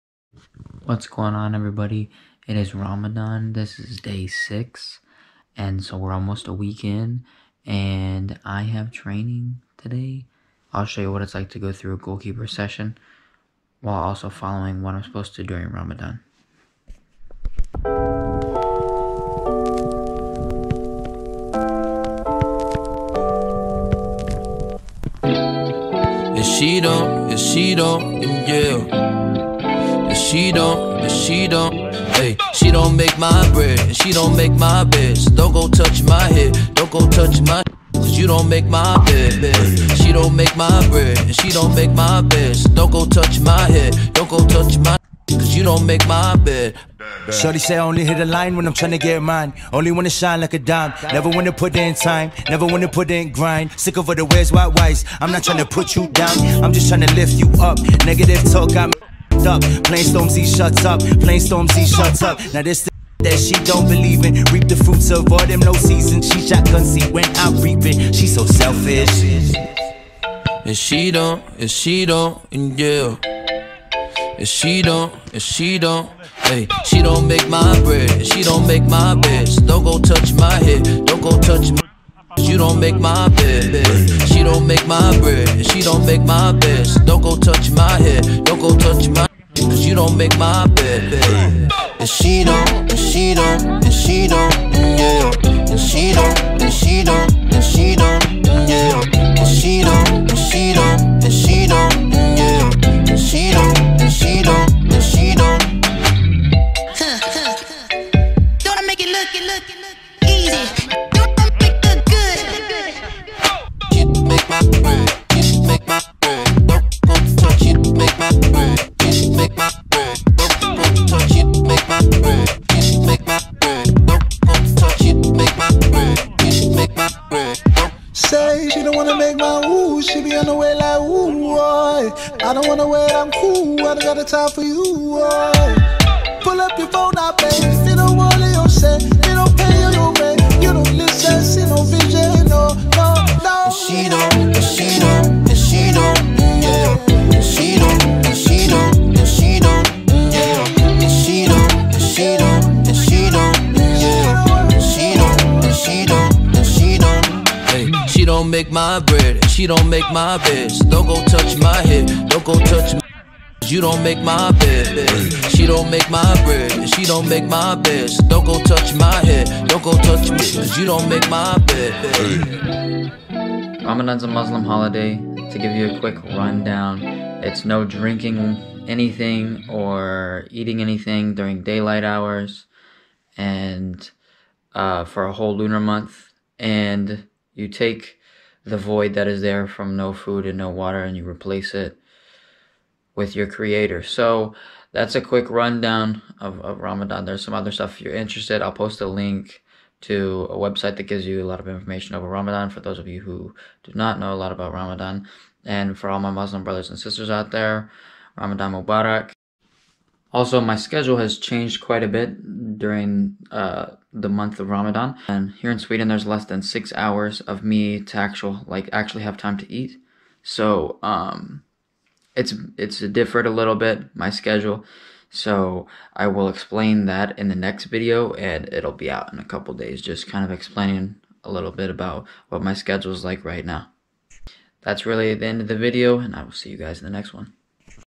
Goalkeeper Session During Ramadan
Here is an insight of what a goalkeeper specific training session looks like. I am currently fasting since it is Ramadan. We are now over a week through Ramadan. My training schedule, diet, and daily routine have changed since the beginning of Ramadan. I will explain the changes in the next video.

For now enjoy this video! Thank you everyone for all of the support!

Royce